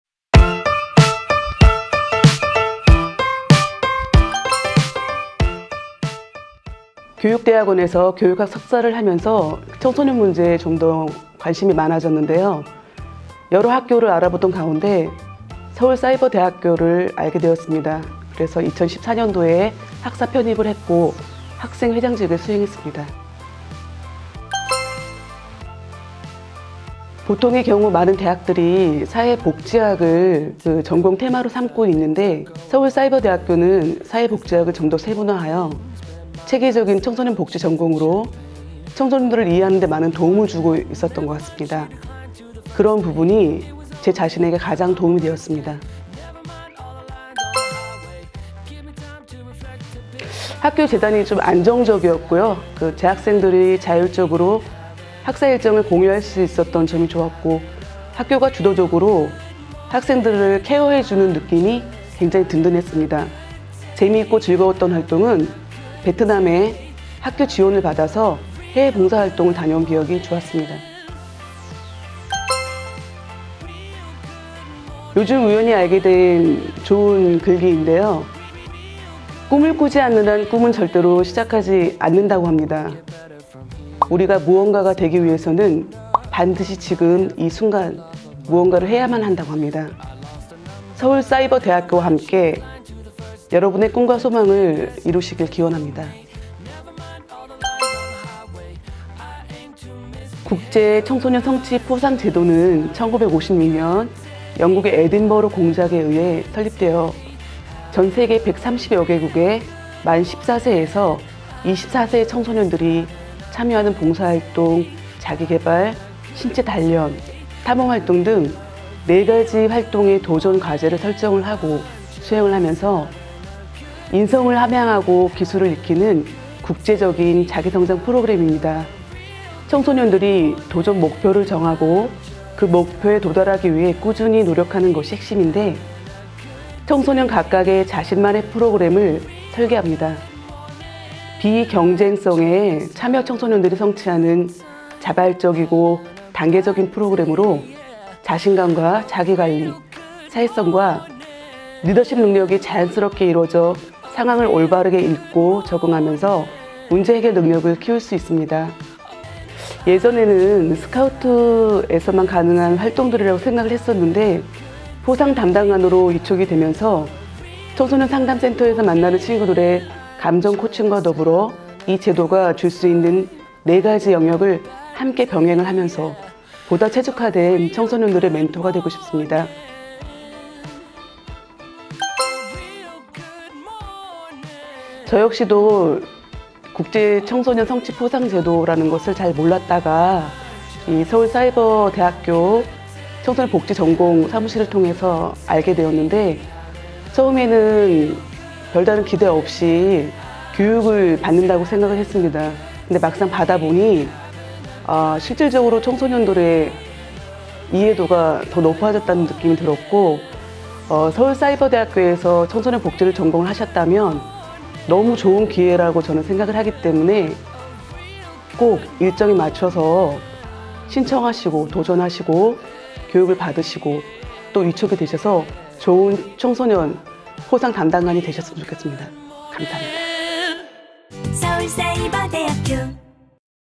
서울사이버대학교 청소년복지전공 강유미 졸업생 인터뷰 / 서울사이버대 신편입생 모집!
'꿈을 꾸지 않는 한 꿈은 절대로 시작하지 않습니다. 무언가 ‘되기(be)’ 위해서는 반드시 지금 이순간 무언가를 ‘해야(do)’한다'고 얘기하는 서울사이버대 청소년복지전공 강유미 졸업생! 국제청소년성취포상제도에 대해 소개하며 청소년들의 멘토를 꿈꾸고 있는 강유미 졸업생을 응원합니다.

국내 사이버대학 최초로 개설되었으며 특화된 온라인교육을 통해 최고의 청소년복지 전문 인력을 양성하고 청소년복지전공에서 신입생, 편입생을 모집합니다!